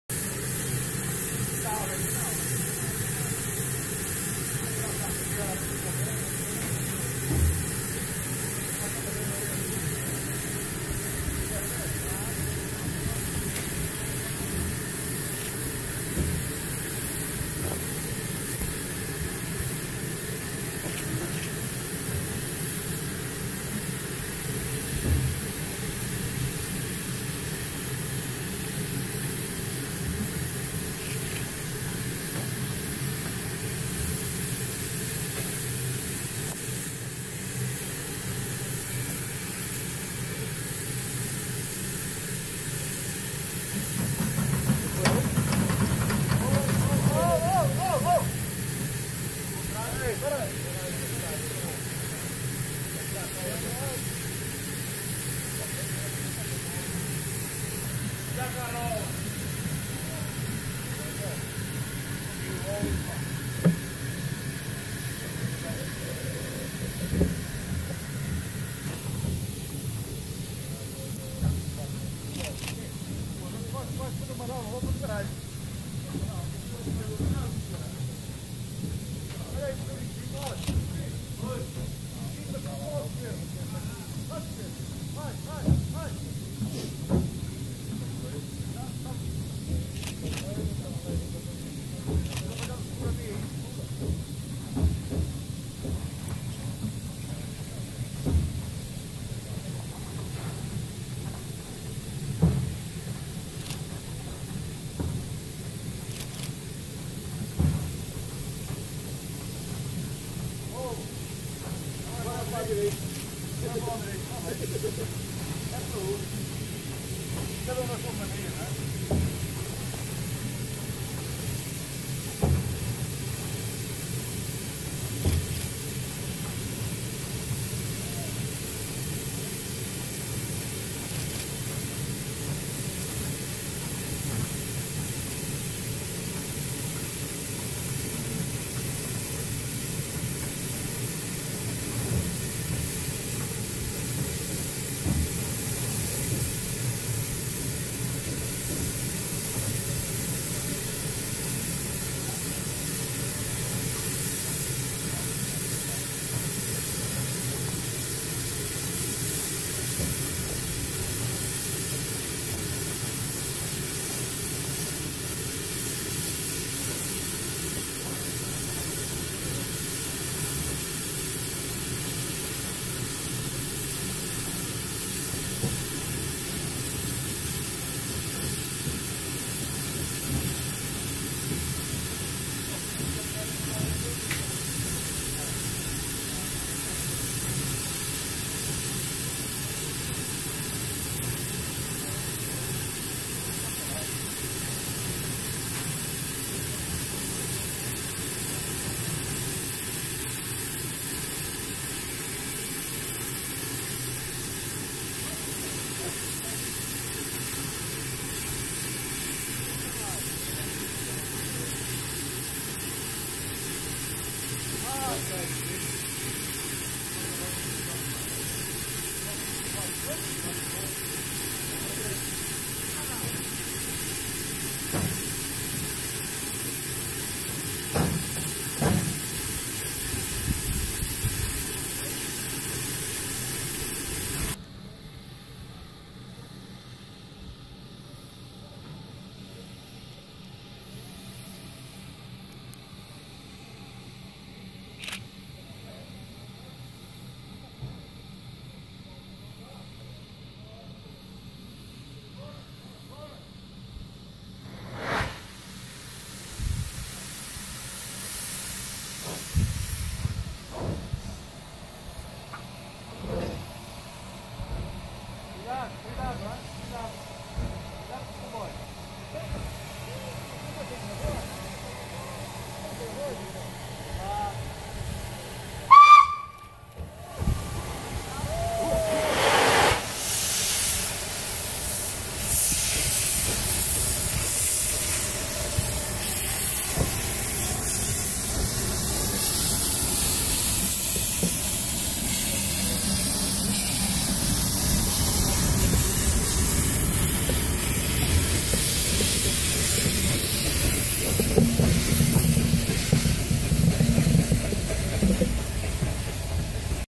CP 0186 na Manobra de Inversão - Tua
Material Circulante: CP 0186
Hora: 16:52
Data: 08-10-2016
Local: Estação do Tua (PK 139 - Linha do Douro)
Serviço: Manobra de inversão da Locomotiva. Integrada no Comboio Histórico 2016.